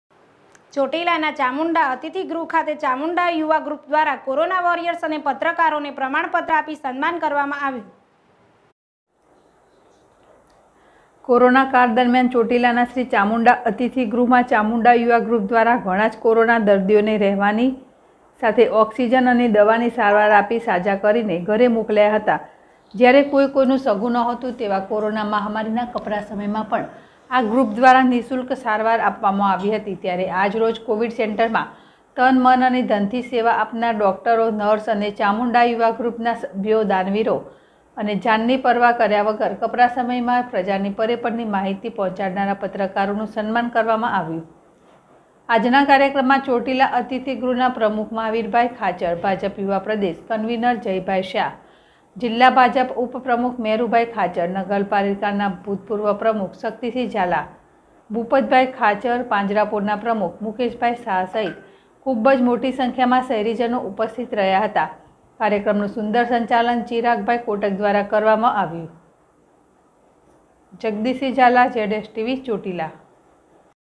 ચોટીલા ચામુંડા અતિથિ ગ્રૂહ મા કોરોના વોરિયર્સ નુ સન્માન કરવામાં આવ્યુ//ZSTVNEWS//
ચોટીલા ચામુંડા અતિથિ ગ્રૂહ મા કોરોના વોરિયર્સ નુ સન્માન કરવામાં આવ્યુ